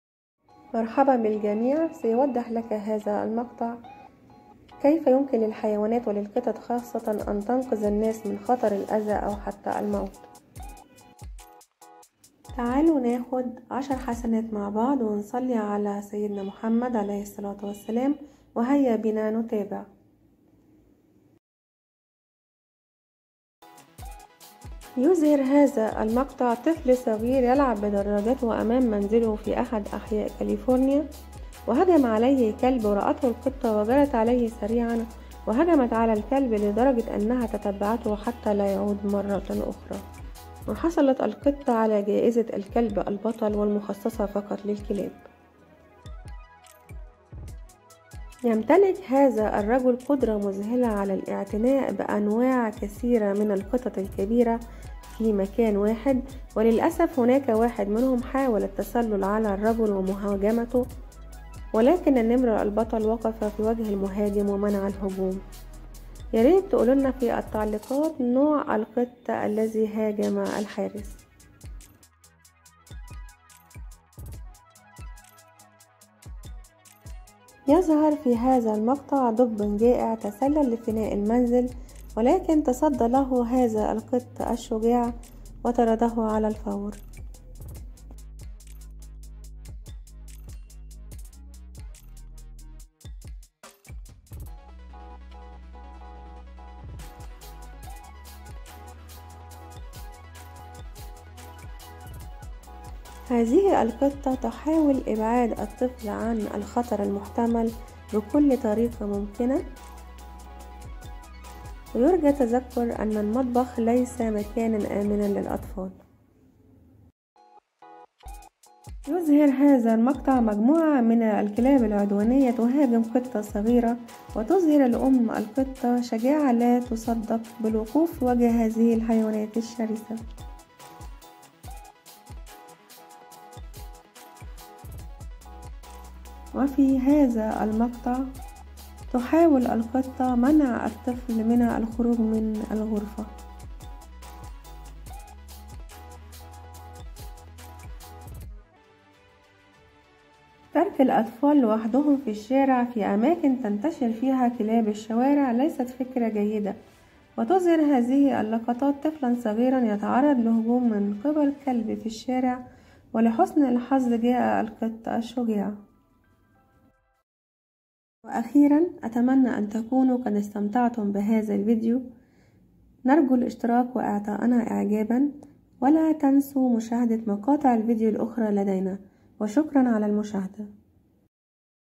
مشاهد# لقطط أنقذت اصحابها من الموت#قطط
سيوصح لك هذا المقطع كيف يمكن للحيوانات والقطط خاصة ان تنقذ الناس من الاذي او حتي الموت وممكن تفعل أي شئ للدفاع عن مالكها
مشاهد لقطط تنقذ اصحبها من الموت المؤكد
القطة تارا
لم تتمكن القطة "تارا" من الوقوف ساكنة أمام كلب ضخم يهاجم صديقها المقرب "جيريمي" فهاجمت الكلب ومنعته من إيذاء صديقها.
وكان "جيريمي تريانتافيلو" البالغ من العمر 4 سنوات، يلعب على دراجته أمام منزله عندما هاجمه كلب الجيران، وضربه بقوة وجره على الأرض، فقفزت "تارا" وهاجمت الكلب وأبعدته عن صديقها، وتدخلت والدته في الوقت المناسب لتنقذ ابنها وقطتهم من الكلب.
وذكرت الأم أن "جيرمي" حصل على 10 غرز ولولا "تارا" كان الأمر سيكون أسوأ بكثير.